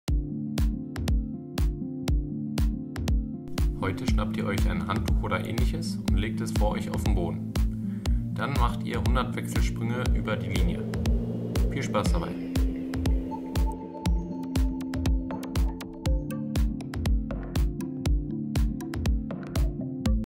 Januar - Challenge Grundschulliga
Wir bauen uns eine Linine und nutze diese für Sprünge :-)